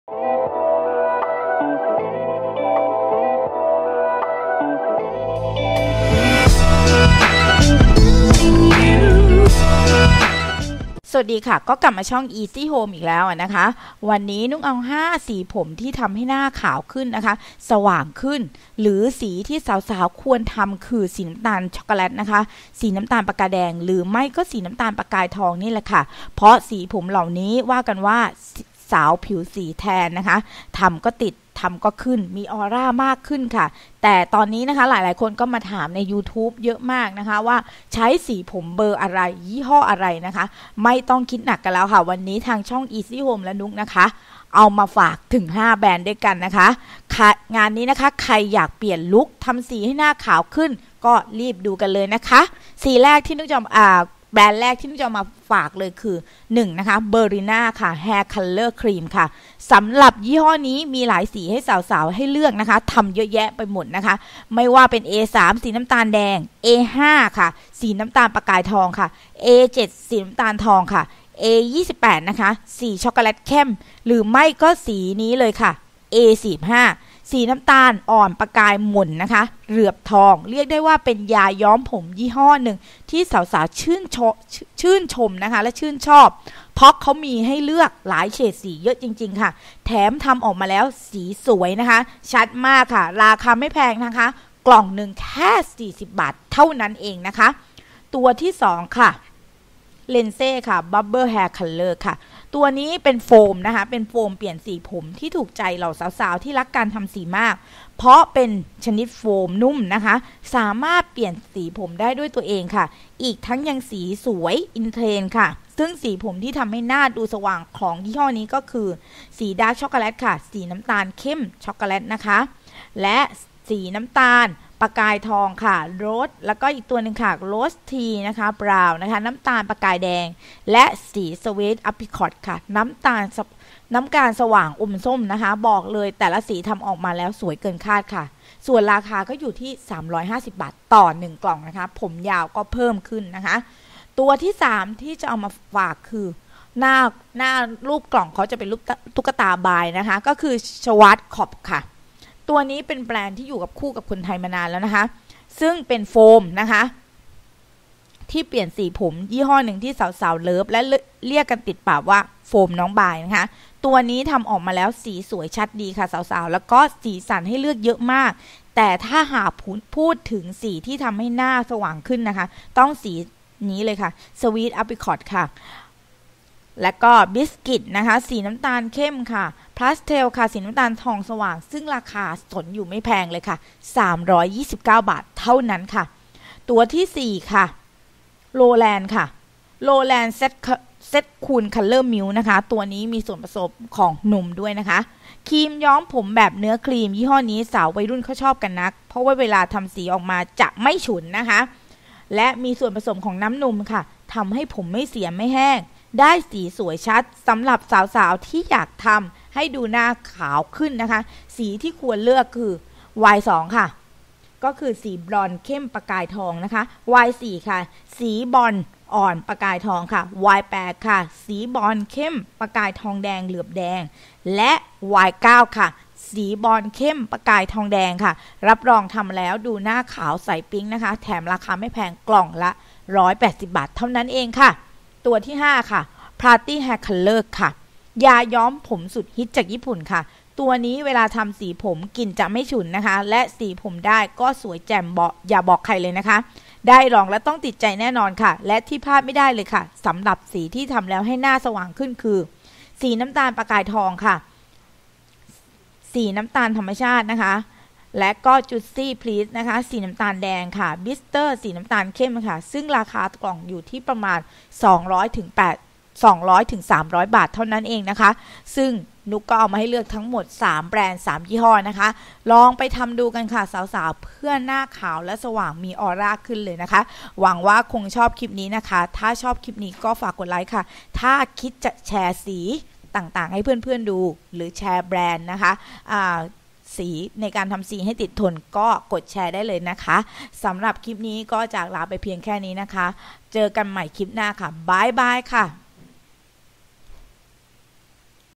5 สีผมสวยๆที่ทําให้หน้าขาว แค่เปลี่ยนสีผม ชีวิตก็เปลี่ยน สีผมที่เหมาะกับสีผิว ทำแล้วผิวขาวใส
5 สีผมสวยๆที่ทําให้หน้าขาว แค่เปลี่ยนสีผม ชีวิตก็เปลี่ยน สีผมที่เหมาะกับสีผิวหาซื้อง่าย